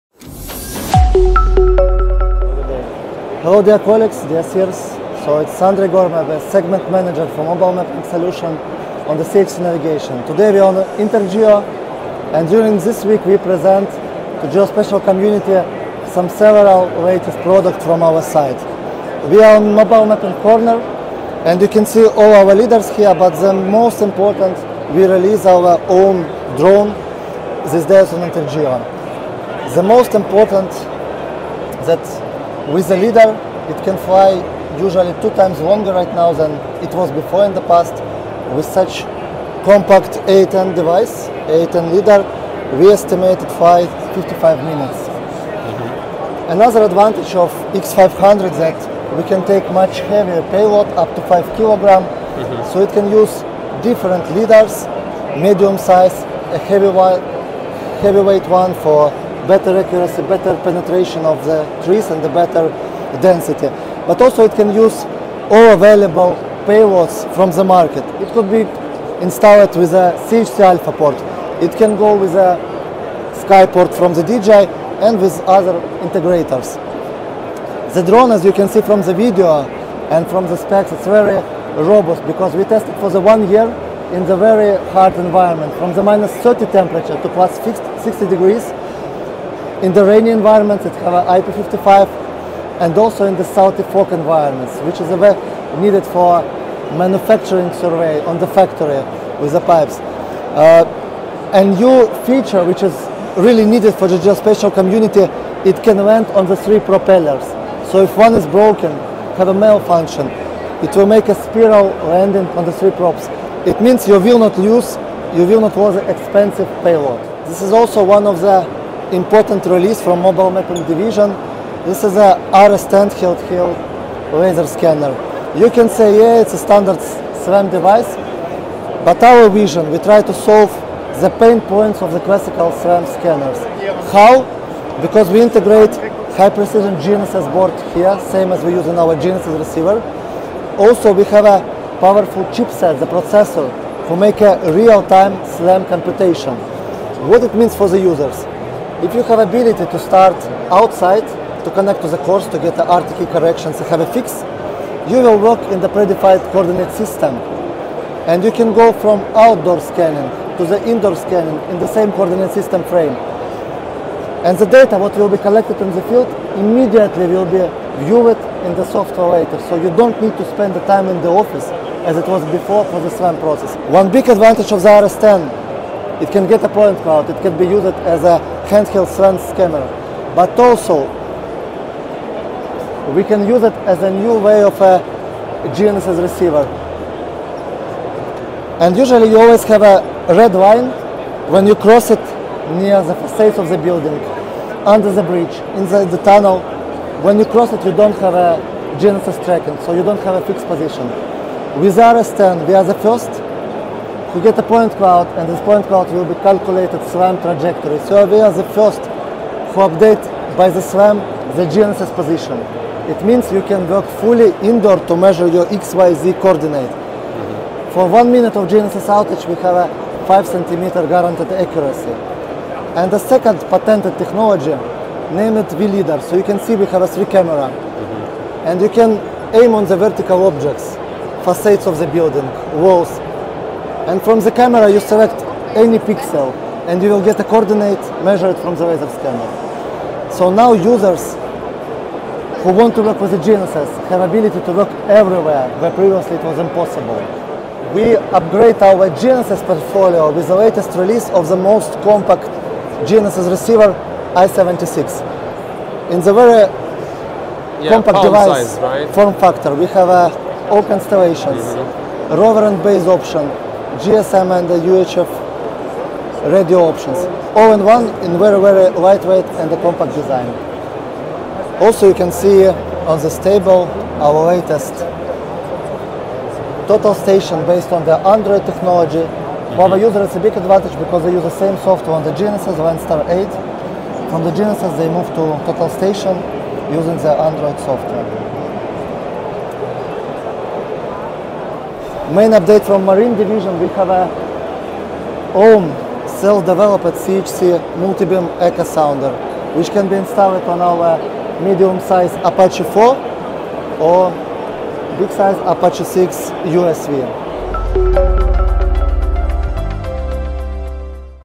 Unveiling CHC Navigation’s Drone: A New Era in Aerial Surveying!
In this video from INTERGEO 2024, we host Andrei Gorb to discuss CHC Navigation's latest drone, built for precision and efficiency in aerial surveying. Learn about its advanced features and how it's designed to improve data collection and streamline workflows for surveyors. Don't miss this overview of the future of drone technology in geomatics!